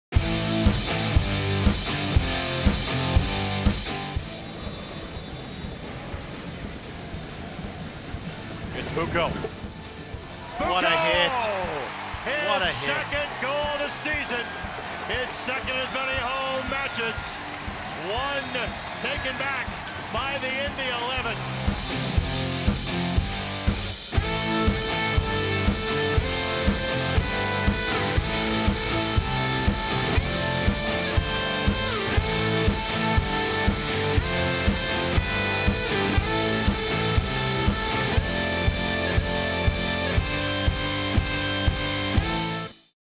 NASL POTW | Week 2 - Nemanja Vukovic
Indy Eleven defender Nemanja Vukovic takes home NASL Play of the Week honors for Fall Week 2 after producing a gorgeous free kick in front of the Brickyard Battalion.